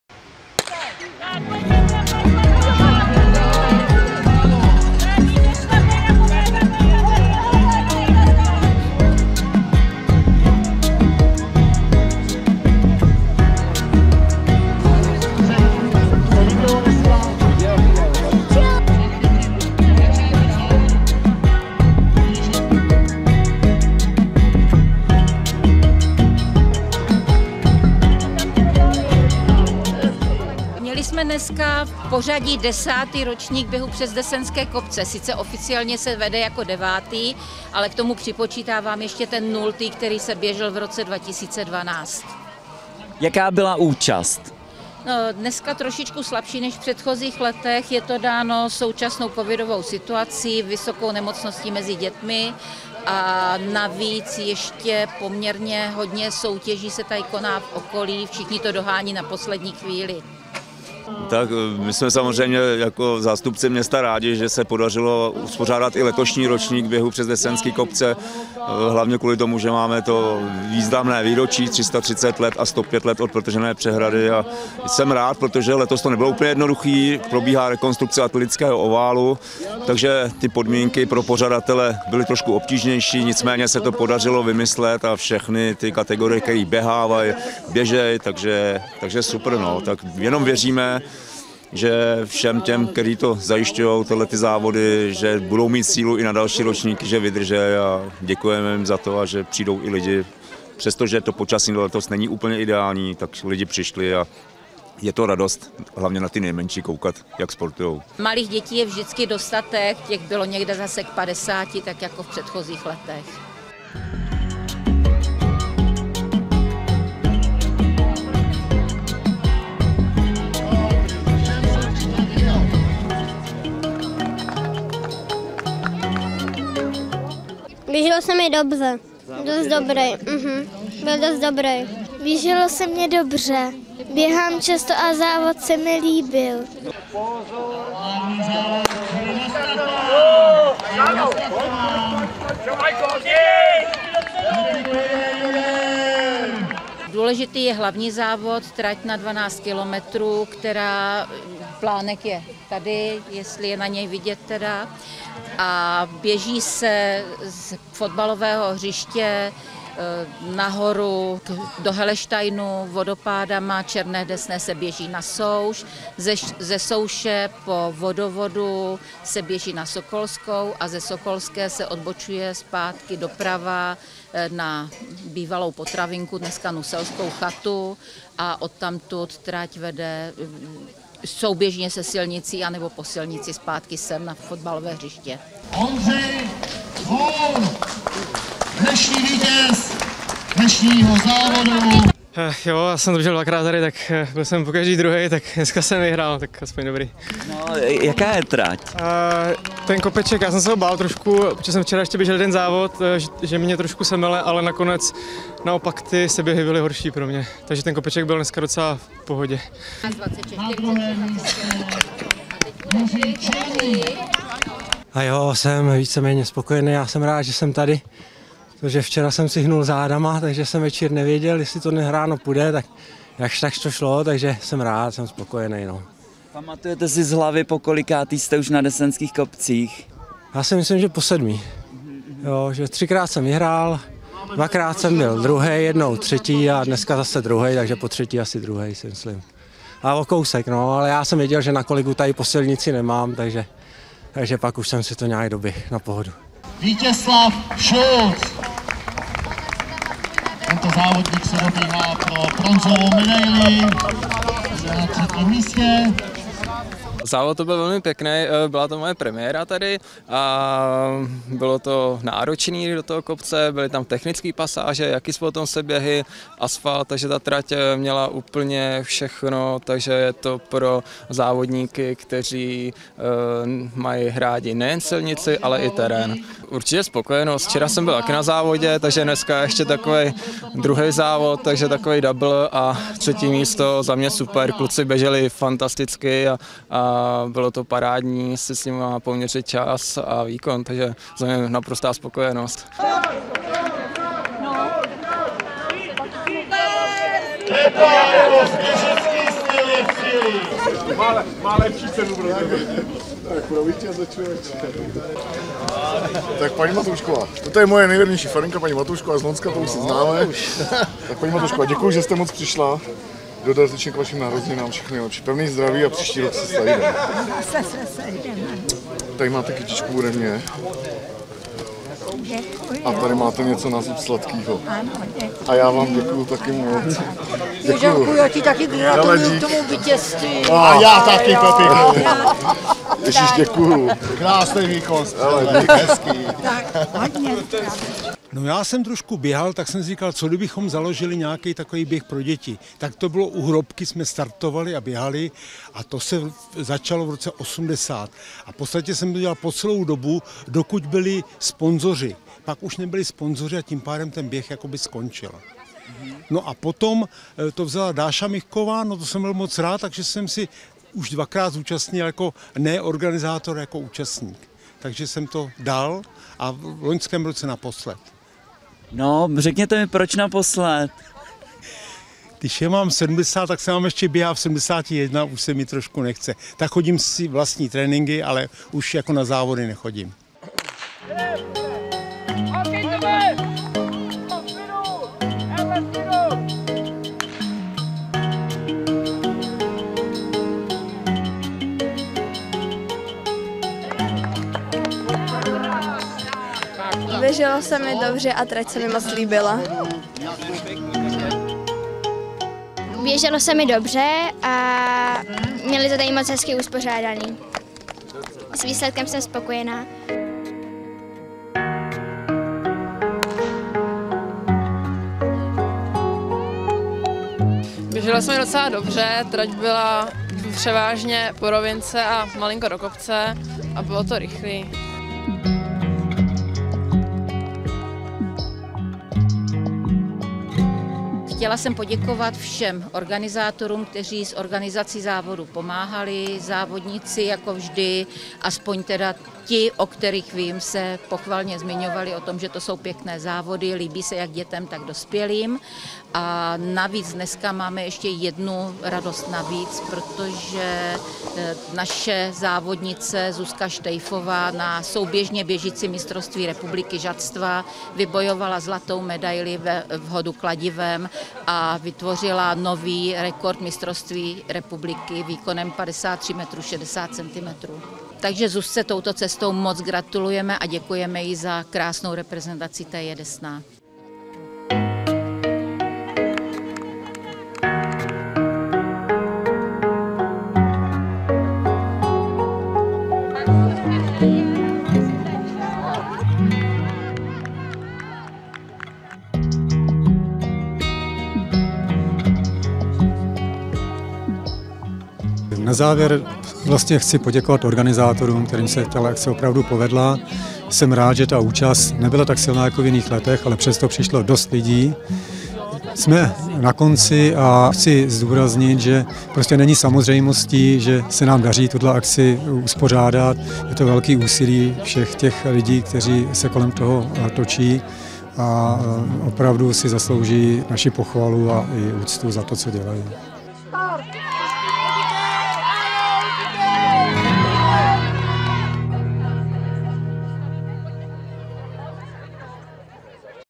Přes desenské kopce 19. 9. 2021
TJ Desná a město Desná uspořádali 9. ročník obnoveného tradičního běhu při příležitosti 105. výročí protržení přehrady na Bílé Desné.